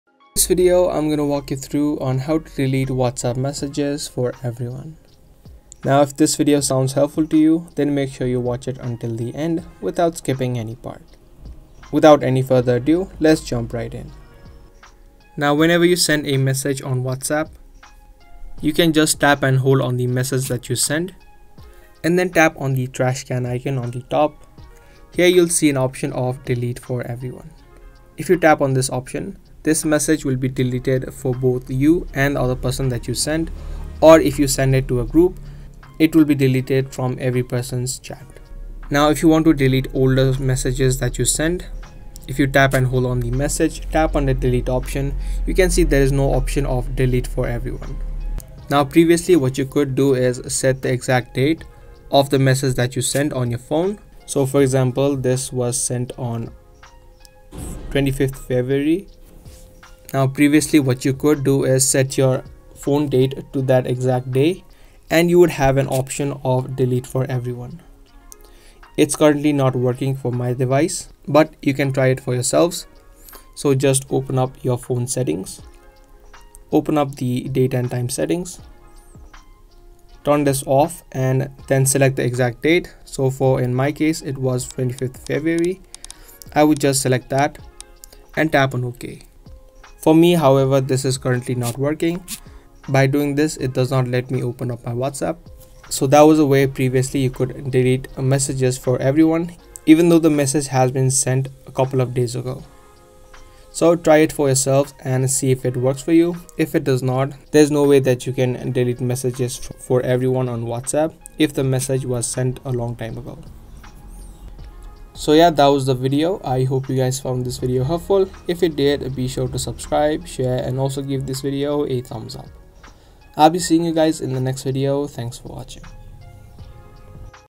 How To Delete WhatsApp Messages For Everyone After Long Time (2023)
In this video I’ll be showing you how to delete messages for everyone on whatsapp. so watch the video till the end so that you won’t miss any steps on how to delete your whatsapp messages for everyone even after long time in iPhone or Android.